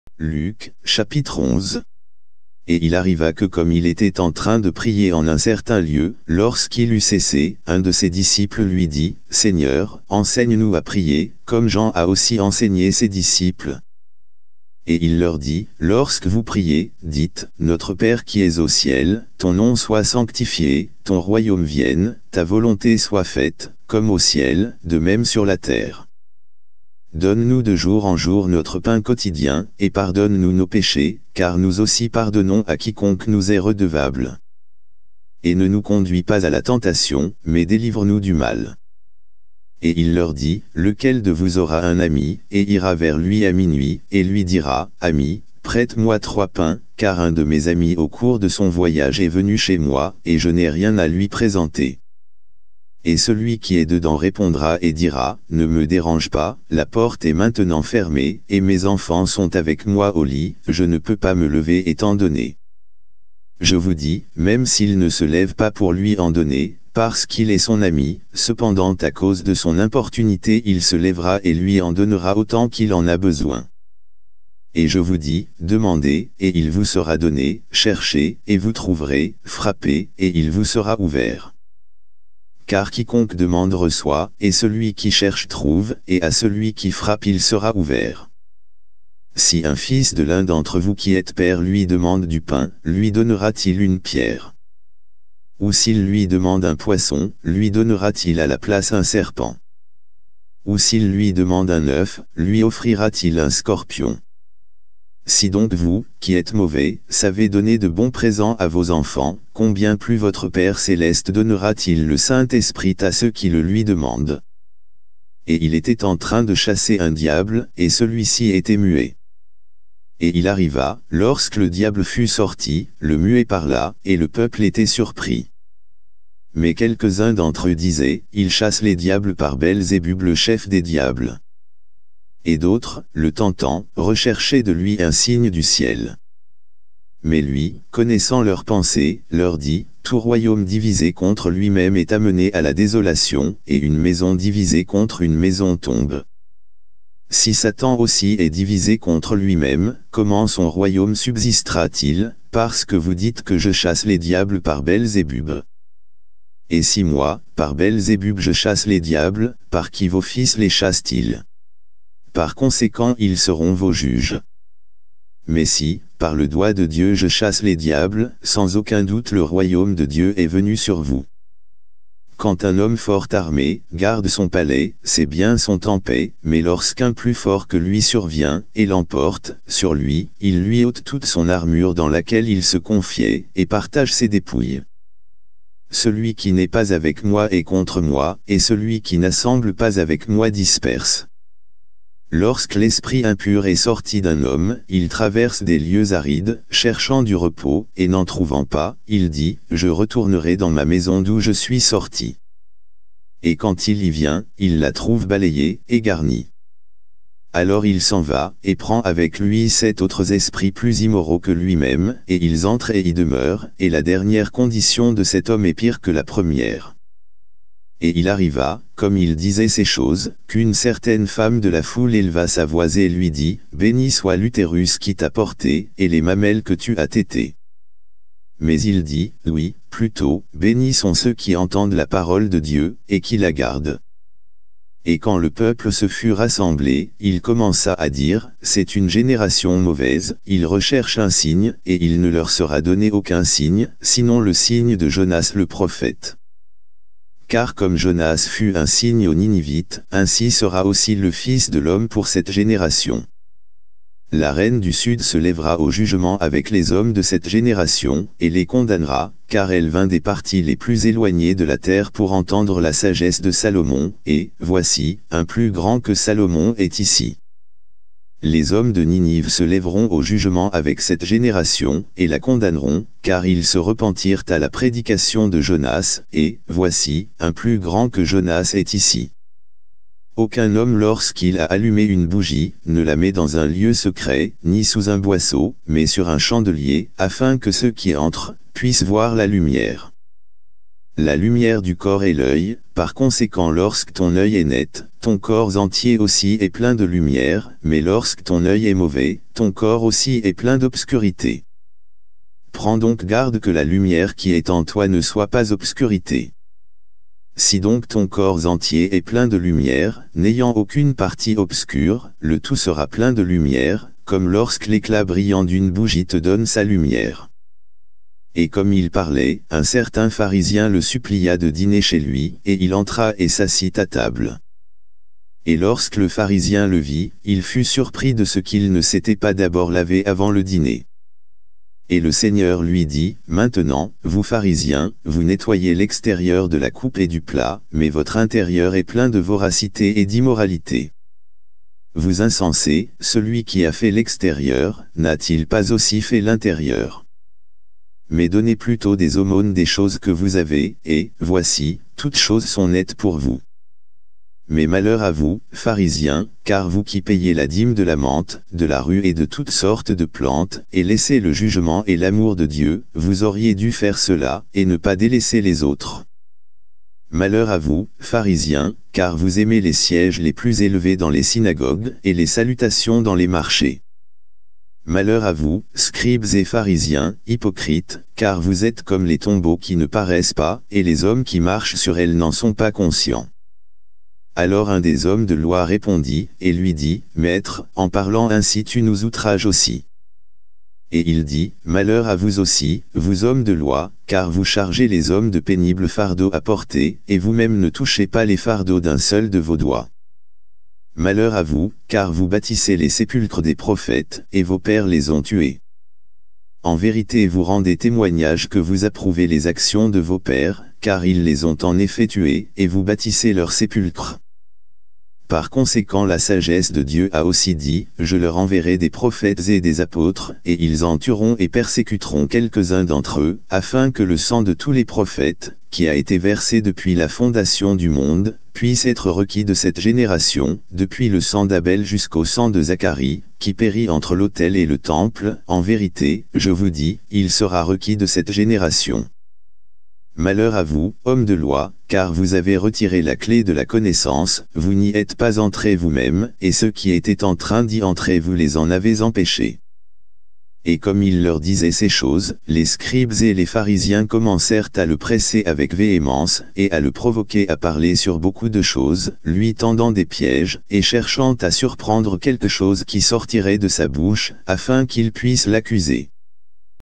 Évangile de Luc Chapitre 11 - La Bible Version King James En Livre Audio Français
L'évangile de saint Luc, chapitre 11, dans la version française de la Bible King James, à écouter en entier en ligne, selon le nouveau testament en audiobook français gratuit au format MP3.